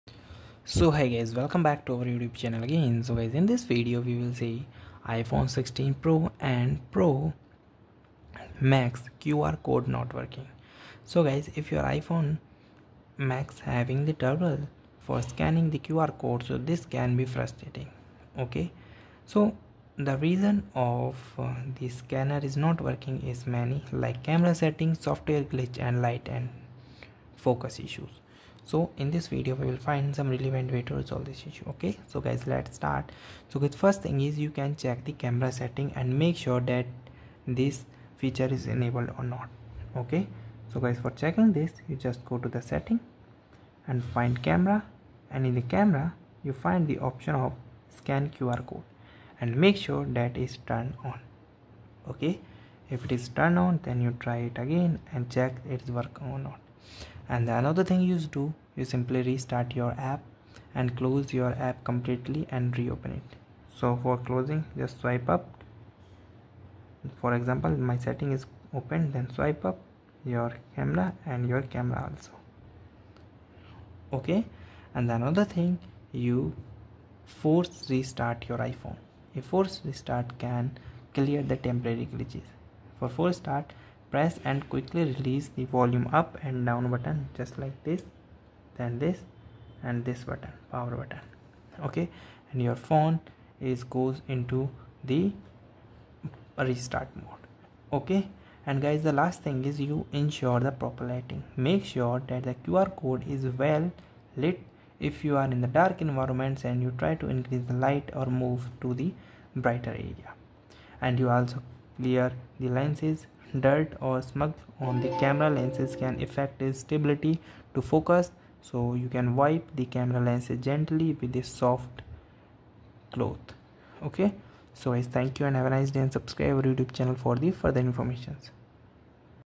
iPhone 16 Pro Max/16 Pro QR Codes Not Working: Here's How To Fix
In this informative video, we address the common issue of QR codes not functioning on the iPhone 16 Pro Max and iPhone 16 Pro.

0:20  Reasons
0:33  Fix1  Check Camera Setting
0:57  Fix2  Restart Your App
1:14  Fix3  Force Restset Your iPhone
1:38  Fix4  insure You Are In Proper Lightning